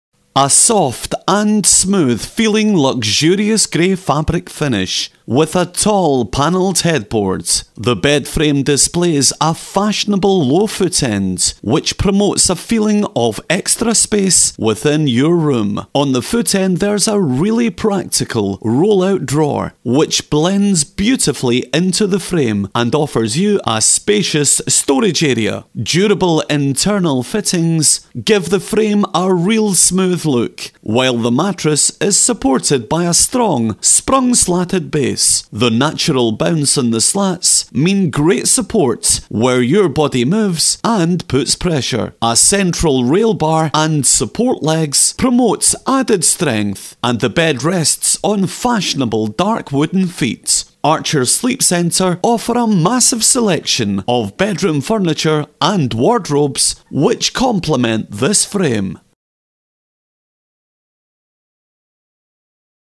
Time Living Atlanta Grey Fabric Bed Frame
a soft and smooth feeling luxurious grey fabric finish with a tall panelled headboard. The bed frame displays a fashionable low footend which promotes a feeling of extra space within your room - on the footend theres a really practical roll out drawer which blends beautifully into the frame and offers you spacious storage. Durable Internal fittings give the frame a real smooth look while the mattress is supported by a strong sprung slatted base - the natural bounce in the slats mean great support were your body moves and puts pressure. A central rail bar and support legs promote added strength. Archers sleep centre offer a massive selection of bedroom furniture and wardrobes which compliment this frame.